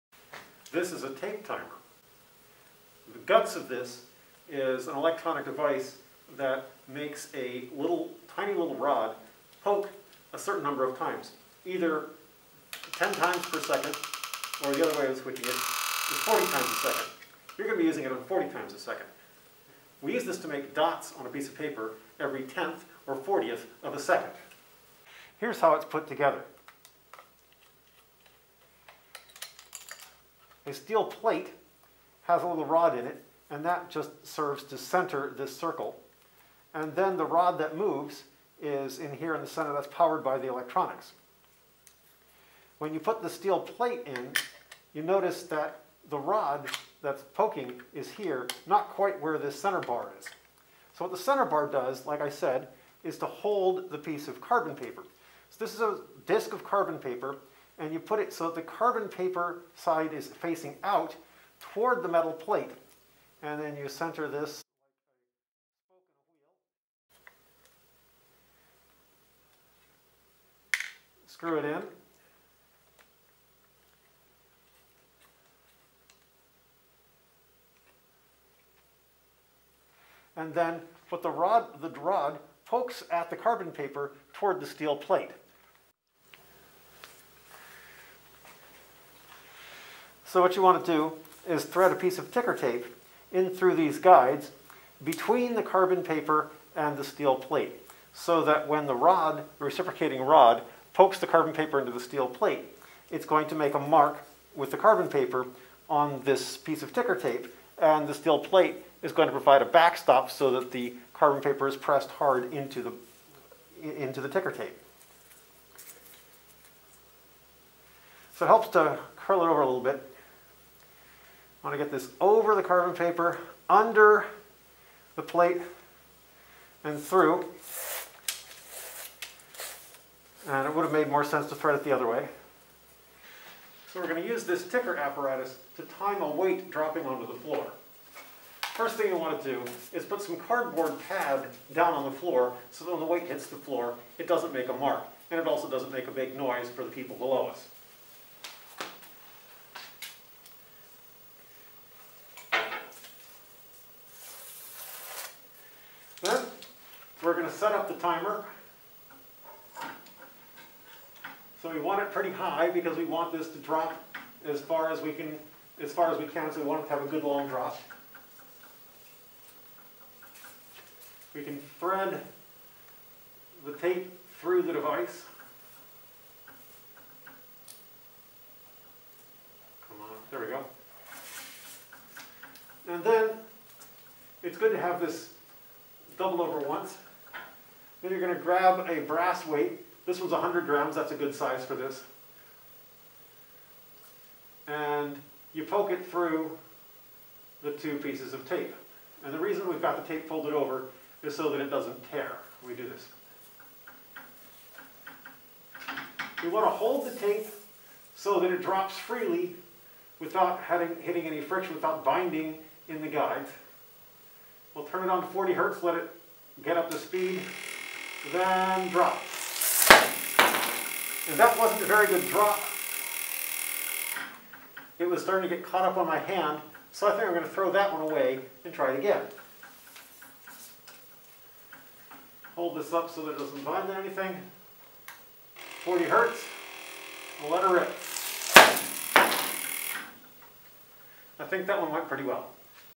Tape timer
Using the Pasco tape timer to track motion.